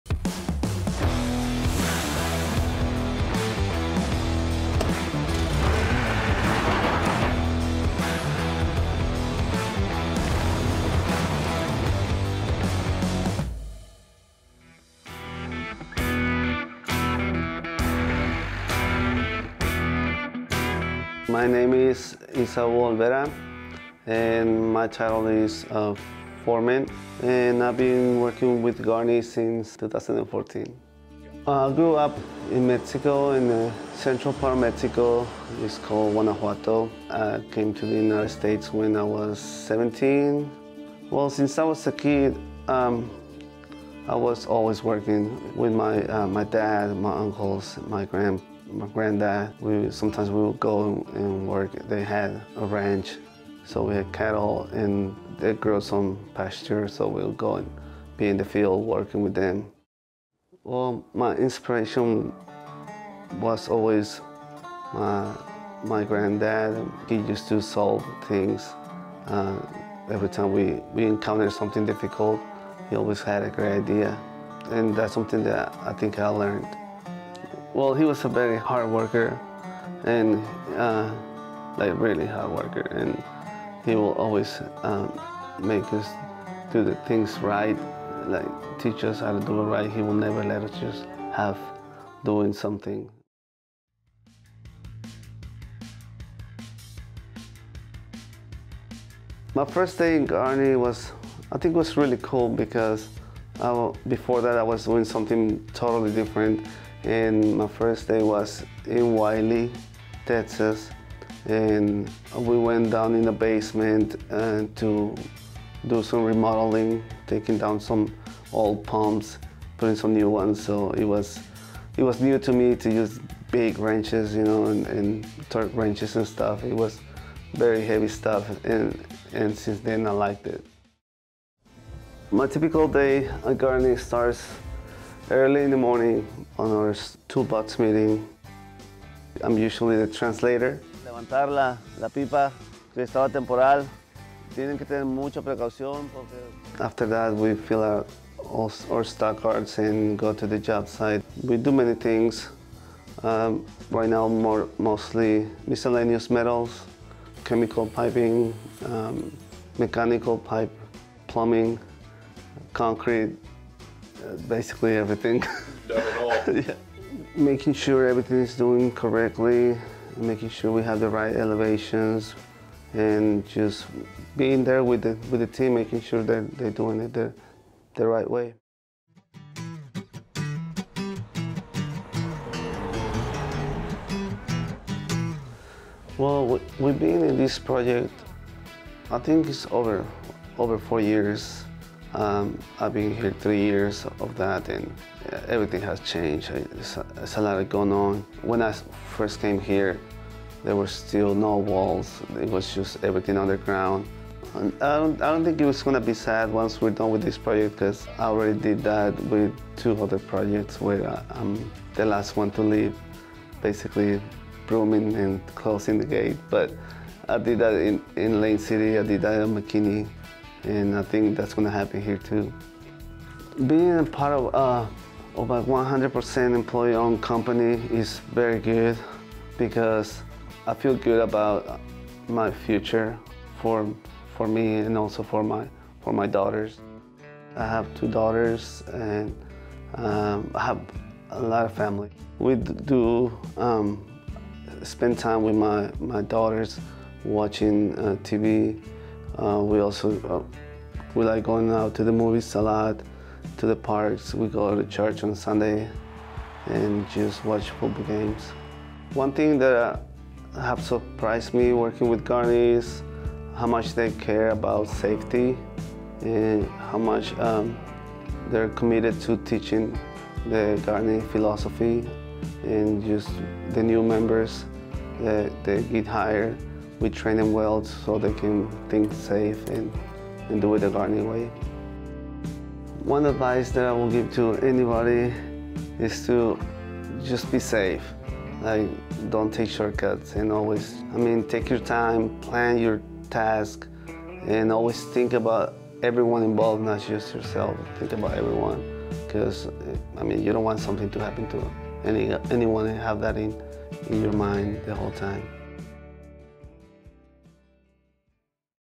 Featured Employee-Owner - Esau Olvera
Our featured employee-owner for the first quarter of 2023 is Esau Olvera, a plant Foreman who has worked on projects throughout Texas.

Esau excels with his crew members to make safety their top priority every day. Difficult tasks and challenging working conditions are never a problem, but an opportunity to excel. Esau is a valuable member of any project team he is a part of, and he is willing to take on any challenge presented to him. His attention to detail is uncanny and he strives to instill this trait in his crew.